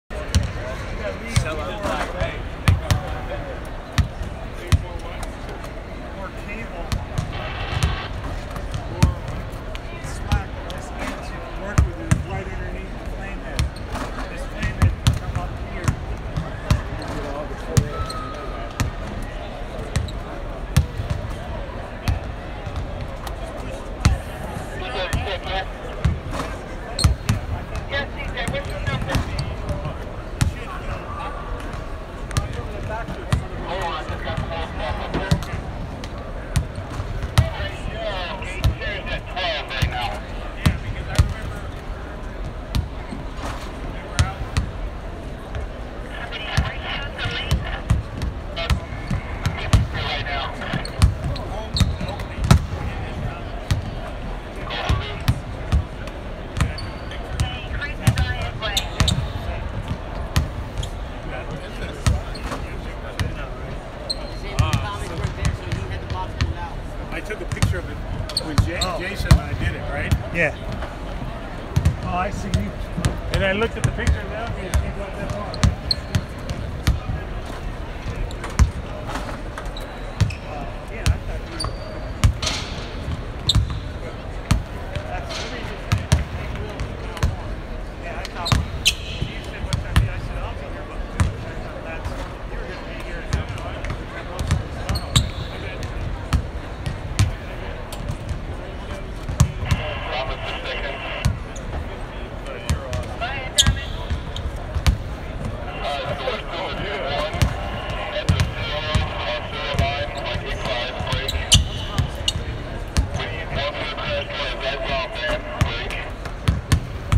Alfonzo McKinnie (out, foot) pregame workout before Warriors (15-9) vs Atlanta Hawks
From Golden State Warriors (15-9) pregame before Atlanta Hawks, at State Farm Arena in Atlanta, GA.